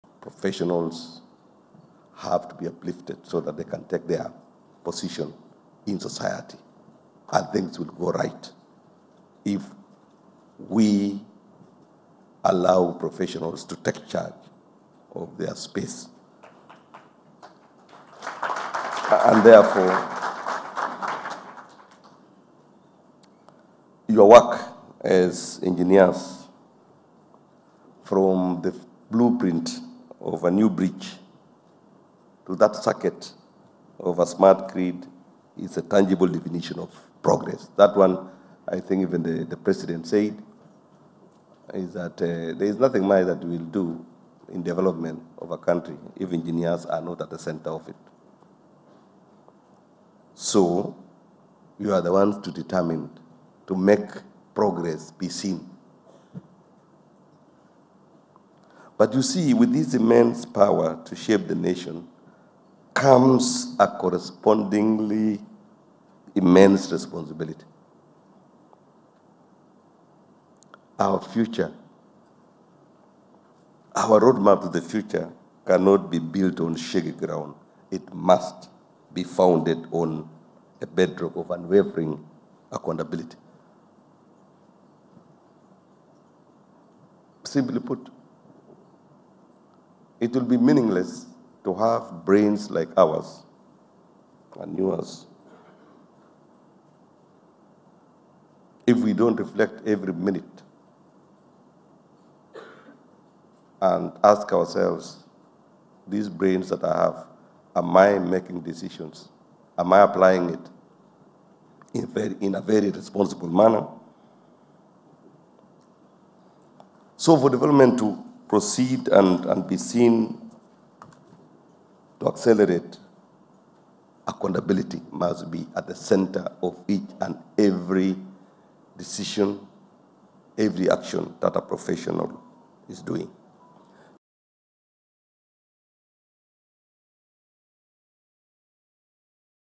Preparing engineers for the future - IEK International Convention
Felix K. Koskei the Chief of Staff and Head of Public Service: There can be no meaningful development without engineers at its centre. With the power to shape the nation comes an equally immense responsibility. Our roadmap for the future cannot be built on shaky ground; it must rest on a bedrock of unwavering accountability.